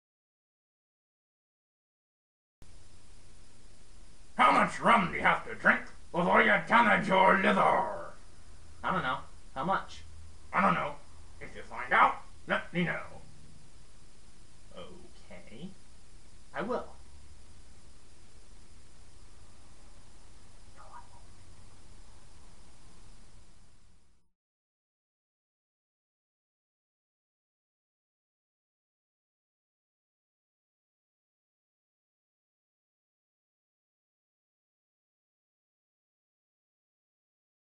Capt. Whitebeard's Joke For The Day (03-17-15)
Capt. Whitebeard's St. Patrick's Day joke. I hope you like this video. Enjoy.